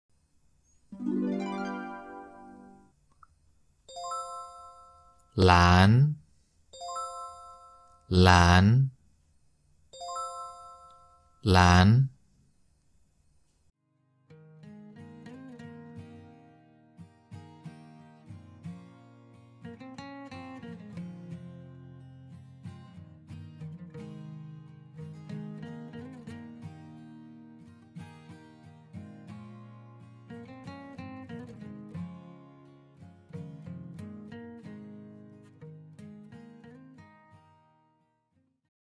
How to Say Your Name LANG in Chinese?
*Formal* Chinese translation of your name LANG. This video shows how native Chinese speakers pronounce and write it (the stroke sequence in Chinese).

-- topic suggestion
-- constructive feedback
-- quick questions